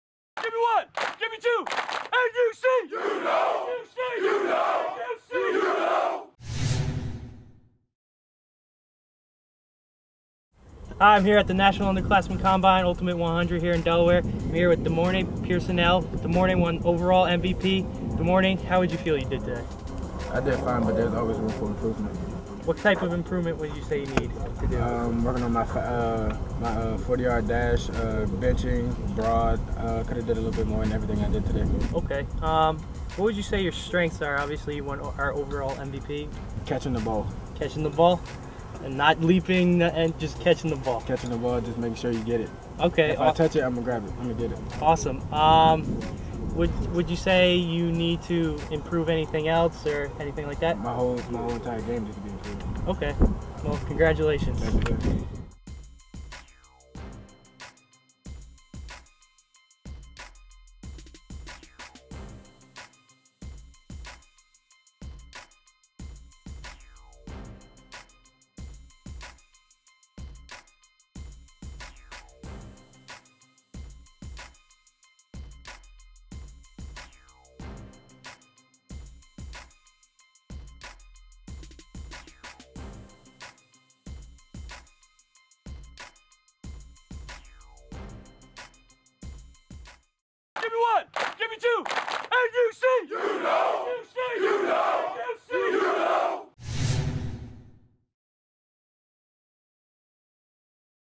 NUC - Demornay Pierson-El - U100 East 2012 - 10th Grade Overall MVP - National Underclassmen
Watch our interview with Demornay Pierson-El, the 10th Grade Overall MVP from Lake Braddock High School, talk about his experience and performance at the Ultimate 100 East 2012 camp by National Underclassmen.

Vimeo.com